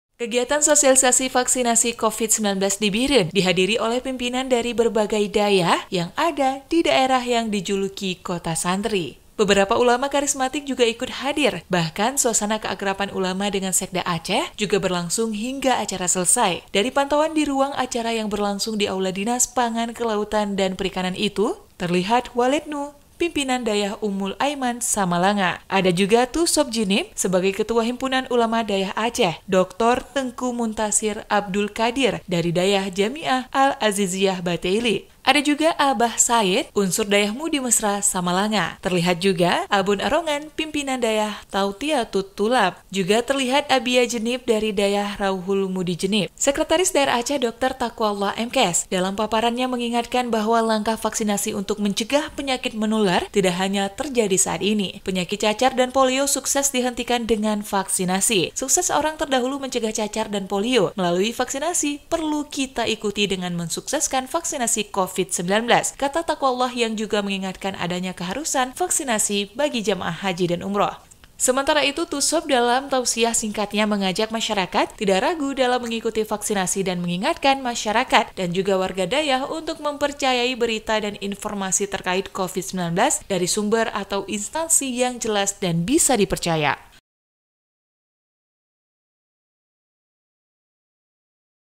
Ulama Kharismatik dan Pimpinan Dayah Antusias Ikuti Sosialisasi Vaksinasi Covid-19
Bireuen - Kegiatan sosialisai vaksinasi Covid-19 di Bireuen dihadiri oleh pimpinan dari berbagai dayah yang ada di daerah yang dijuluki Kota Santri.

Beberapa ulama kharismatik juga ikut hadir bahkan Suasana keakraban ulama dengan Sekda Aceh juga berlangsung hingga acara acara selesai.
Dari pantauan di ruang acara yang berlangsung di Aula Dinas Pangan, Kelautan dan Perikanan itu terlihat Waled Nu, Pimpinan Dayah Ummul Ayman Samalanga

Ada juga Tu Sop Jeunib sebagai Ketua Himpunan Ulama Dayah Aceh. Dr Tgk Muntasir Abdul Kadir dari Dayah Jamiah Al-Aziziyah Batee Iliek.

Ada juga Abah Sayed, unsur Dayah MUDI Mesra Samalanga. Terlihat juga Abon Arongan Pimpinan Dayah Thautiatud Thullab. Juga terlihat Abiya Jeunib dari Dayah Rauhul Mudi Djeunib.
Sekretaris Daerah dr Taqwallah, M. Kes dalam paparan mengingatkan bahwa langkah vaksinasi untuk mencegah penyakit menular tidak hanya terjadi saat ini.
Penyakit cacar dan polio sukses dihentikan dengan vaksinasi. Sukses orang terdahulu mencegah cacar dan polio melalui vaksinasi perlu kita ikuti dengan mensukseskan vaksinasi Covid-19,” kata Taqwallah yang juga mengingatkan adanya keharusan vaksinasi bagi jamaah haji/umrah.

Sementara itu, Tu Sop dalam tausyiah singkatnya mengajak masyarakat tidak ragu dalam mengikuti vaksinasi dan mengingatkan masyarakat dan warga dayah untuk mempercayai berita dan informasi terkait covid-19 dari sumber atau instansi yang jelas dan bisa dipercaya.